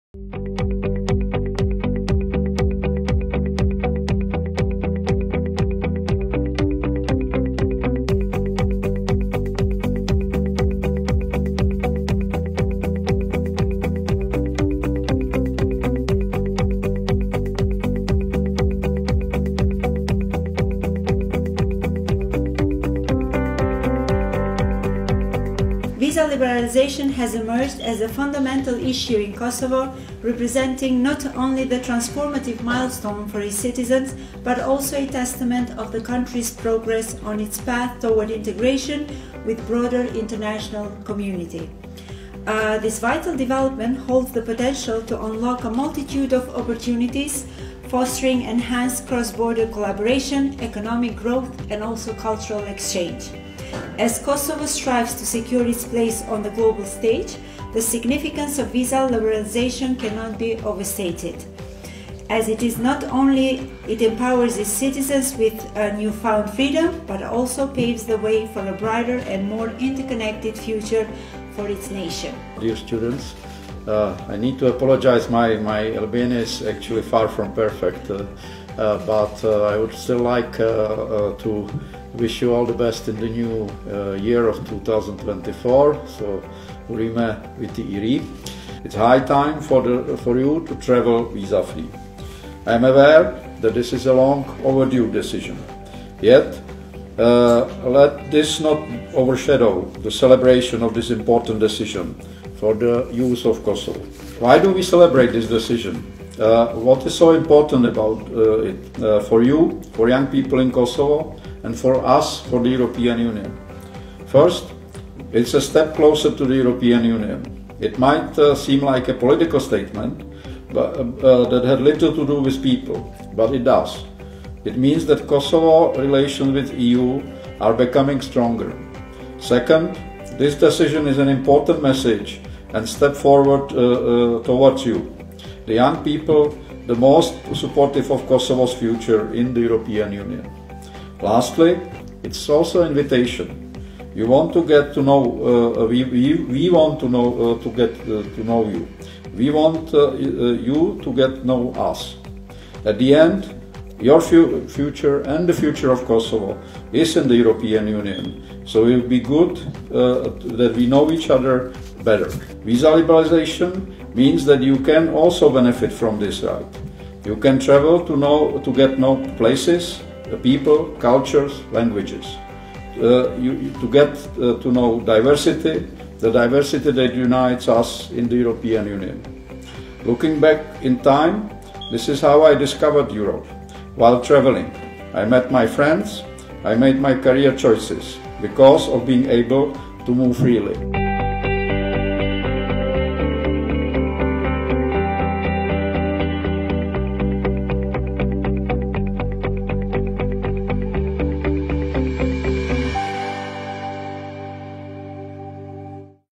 Ligjërata e Ambasadorit të BE-së, Mr. Tomáš Szunyog, në UNI- Universum International College
Video nga ligjërata e Ambasadorit të BE-së 𝐌𝐫. 𝐓𝐨𝐦𝐚́𝐬̌ 𝐒𝐳𝐮𝐧𝐲𝐨𝐠 në UNI- Universum International College powered by Arizona State University🥇
Ambasadori i BE-së 𝐌𝐫. 𝐓𝐨𝐦𝐚́𝐬̌ 𝐒𝐳𝐮𝐧𝐲𝐨𝐠 ka mbajtur 𝐋𝐢𝐠𝐣𝐞̈𝐫𝐚𝐭𝐞̈ 𝐏𝐮𝐛𝐥𝐢𝐤𝐞 për studentët e UNI-Universum International College duke diskutuar mbi çështjen ‘𝑳𝒊𝒃𝒆𝒓𝒂𝒍𝒊𝒛𝒊𝒎𝒊 𝒊 𝑽𝒊𝒛𝒂𝒗𝒆 𝒑𝒆̈𝒓 𝑲𝒐𝒔𝒐𝒗𝒆̈𝒏: 𝑴𝒖𝒏𝒅𝒆̈𝒔𝒊𝒕𝒆̈ 𝒅𝒉𝒆 𝑺𝒇𝒊𝒅𝒂𝒕’.

Ligjërata e ambasadorit Szunyog ofroi një kuptim të plotë të qështjes që rrethon liberalizimin e vizave, duke ofruar një këndvështrim në sfidat dhe mundësitë e ardhshme brenda Bashkimit Evropian.

UNI – Universum International College fuqizohet dhe menaxhohet nga Arizona State University, Universiteti më i madh publik në SHBA.